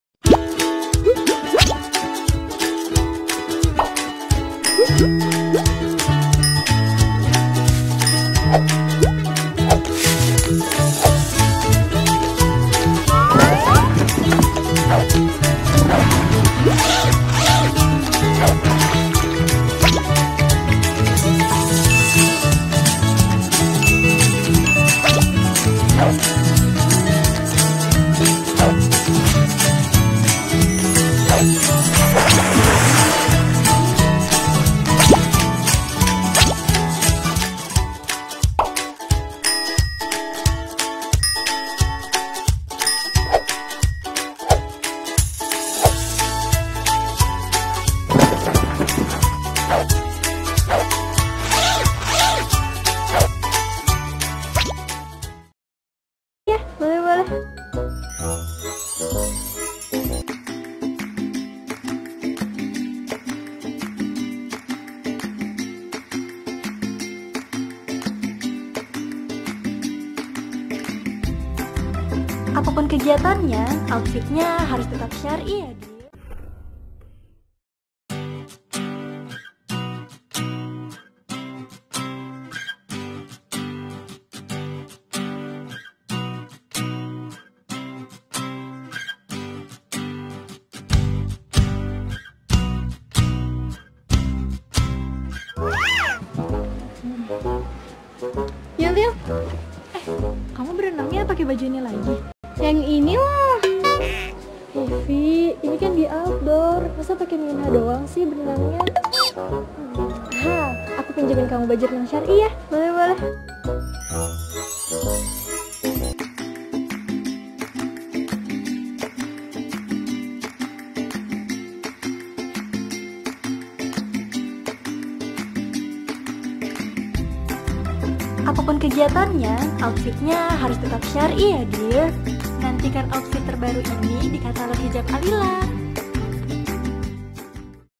Katalog Hijab Alila #2 - WaterDress - Jumsuit Hijab Alila
Yuk dear Intip Tips Berenang ala Alila 😍

READY STOCK! 🎉
Water Dress by Hijab Alila
Dear kamu suka berenang, snorkling, atau bahkan diving ?
Mengarungi dan menyelami dunia bawah laut yang indah nya bisa memanjakan jutaan pasang mata ? 😍 MasyaAllah
Nah kali ini kamu tak usah risau lagi, kamu bisa bergerak bebas tanpa khawatir aurat tersingkap dan pastinya tetap berhijab syar'i dengan :
Warna
~Pink
~Black
Harga
Rp. 235.000,-

JUMPSUIT, 145rb

Bahan Suede microfiber
Dilengkapi dengan zipper dari atas hingga bawah
Khimar menyambung dengan gamis, dengan tambahan aksen tali agar rambut mu tak mengintip intip 😄 .
Yukkk dear segera dapatkan dengan cara hubungi kami ya 😘

Yuk dear buruaaan order!! sebelum kehabisan. Karena stok sangat terbatas 😂😂😂